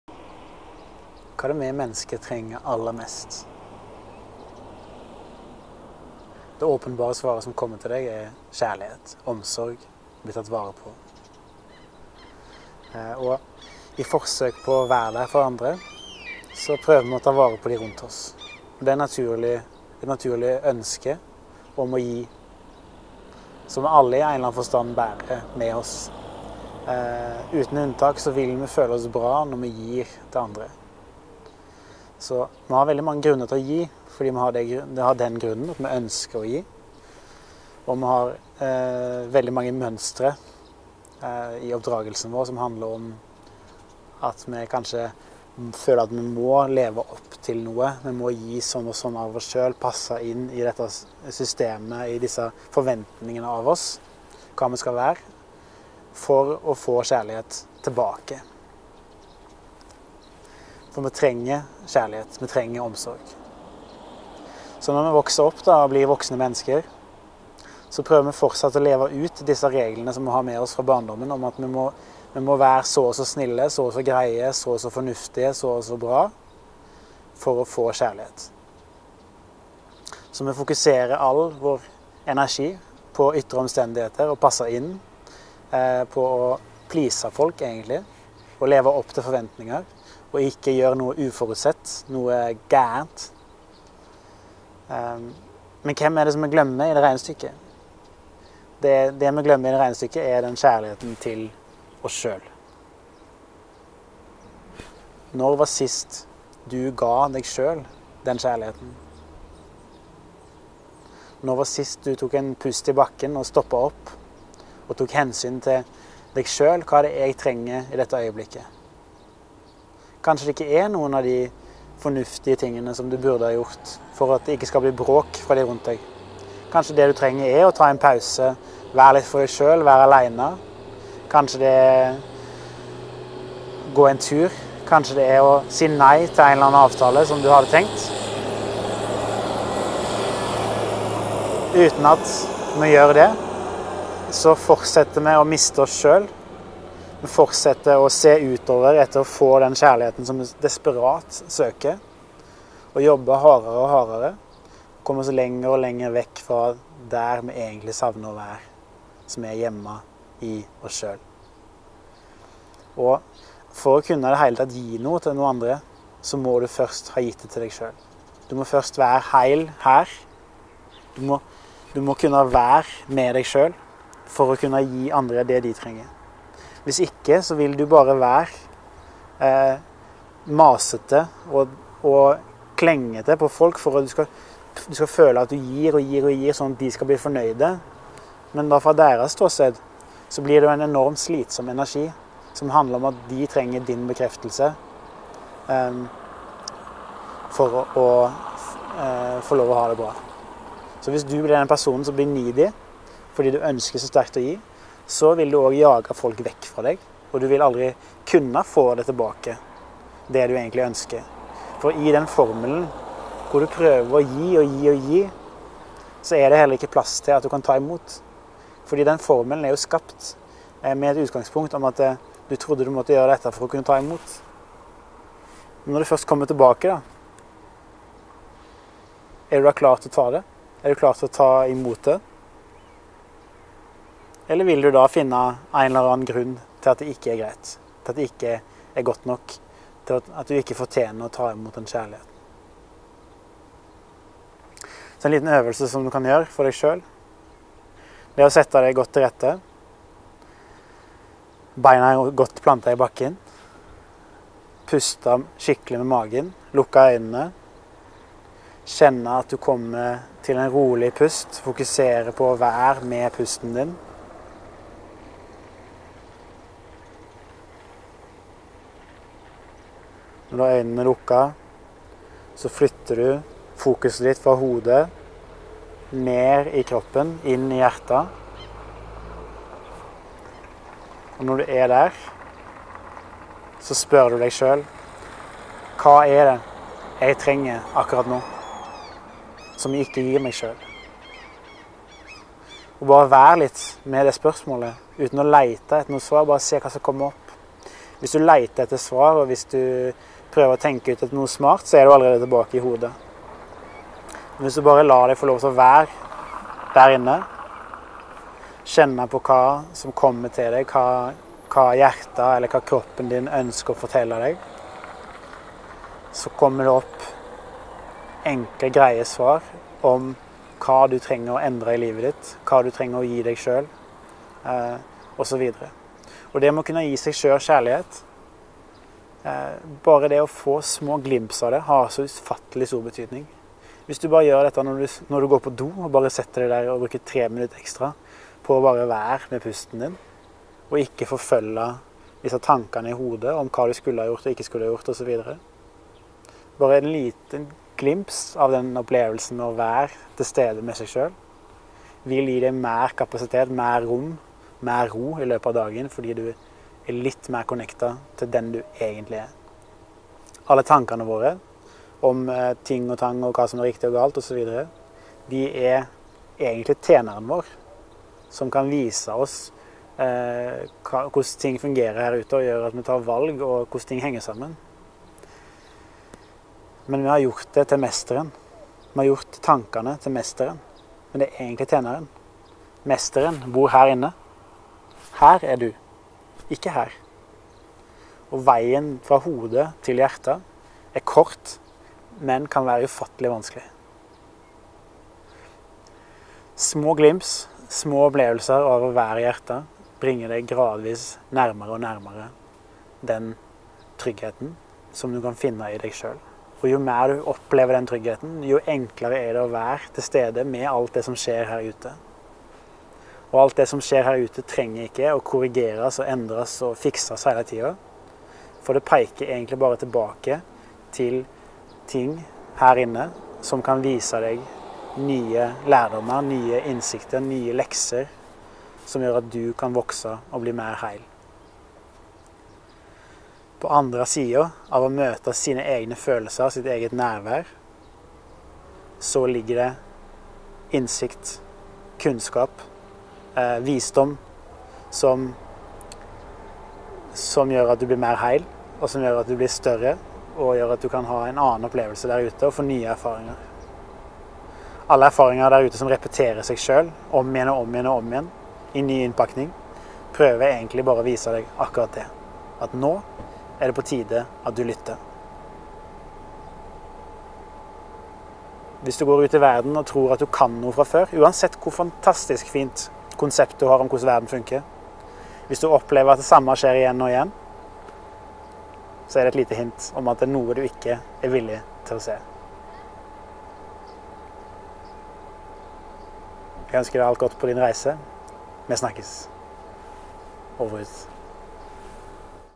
Hverdagstips #31 - Hva trenger du aller mest?
Ta vare på deg selv, kompis. Kjærlighet er ikke en byttehandel.